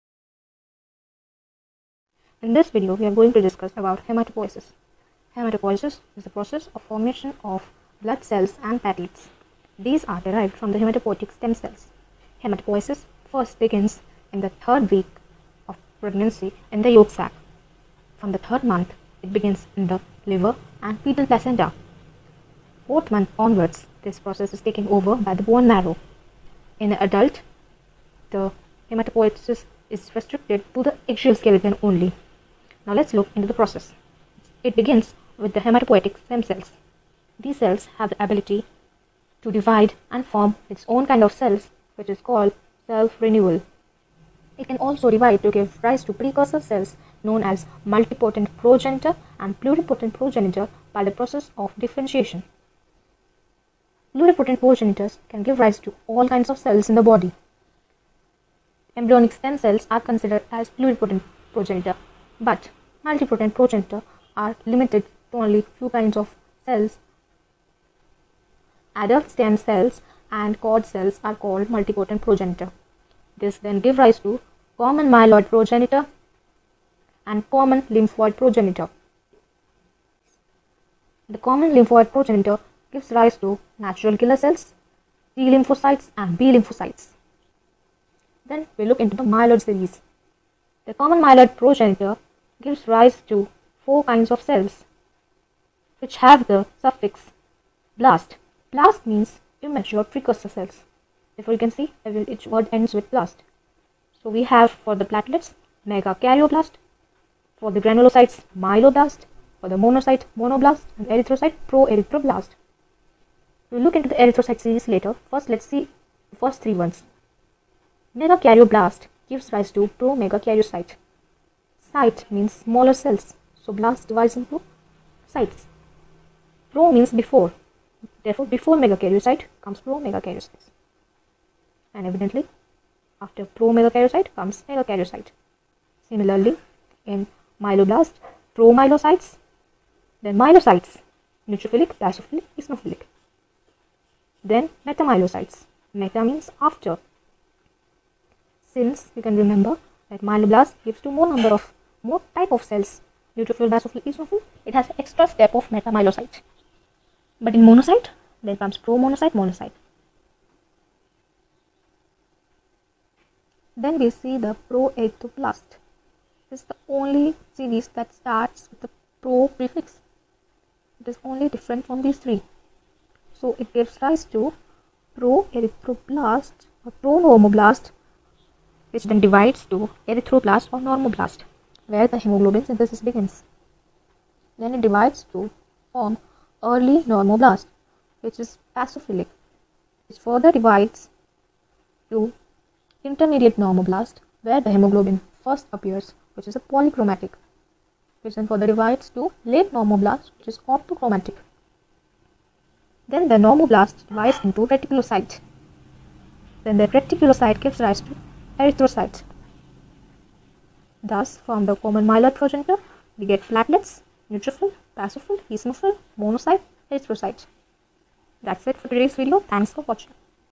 Hematopoiesis: Easy To Remember!
This is a must know in physiology and pathology. How RBCs, WBCs & platelets and other blood cells are formed. Remember this with the help of flow chart.